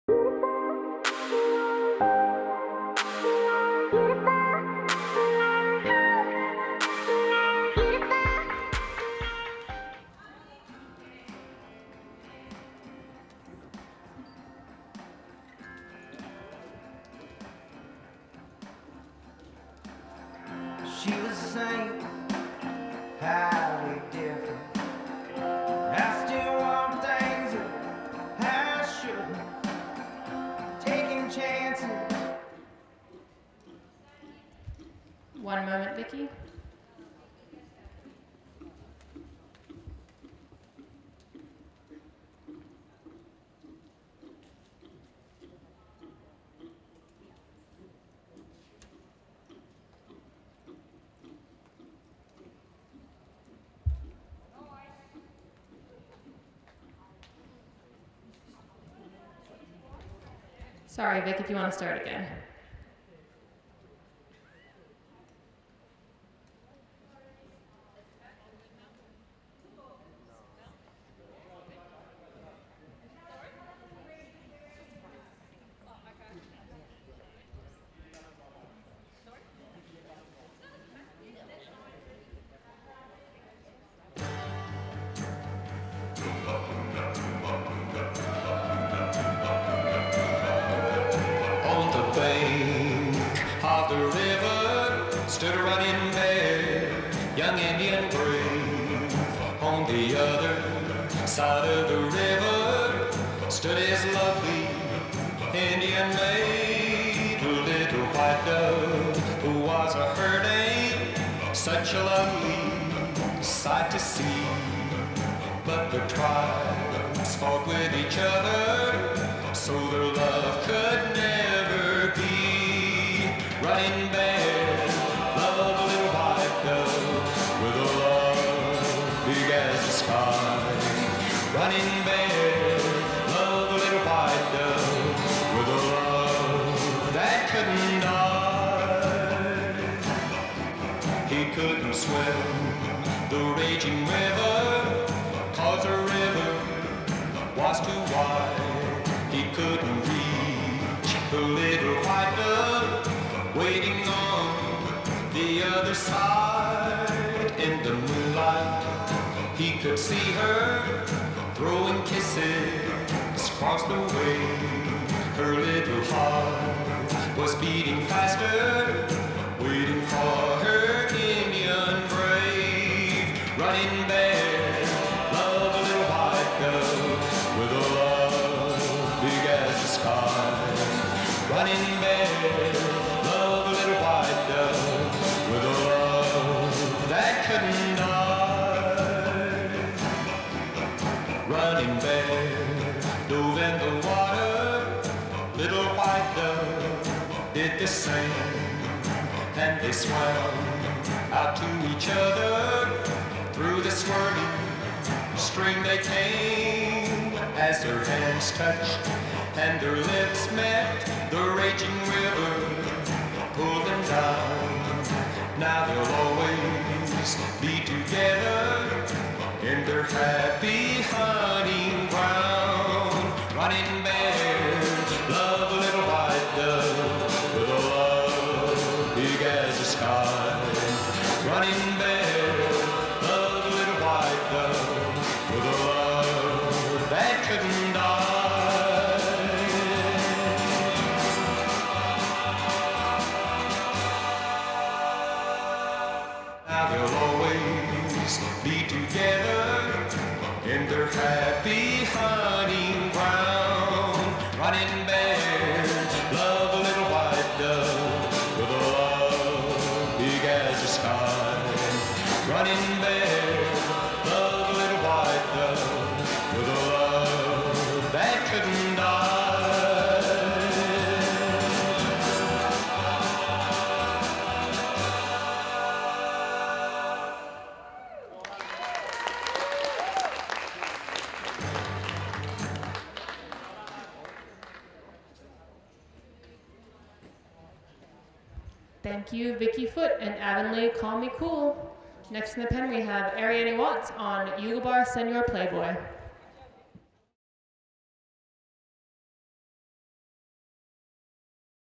2023 Hi Form State Derby Championship Freestyle Champion - Vicki Foot
Congratulations to;

Champion (2 years running)
Vicki Foot riding Avenlee Call Me Cool scoring 71

Reserve Champion
Arianny Watts riding Yulgilbar Senor Playboy scoring 69

Thank you to Paul Newport Video Productions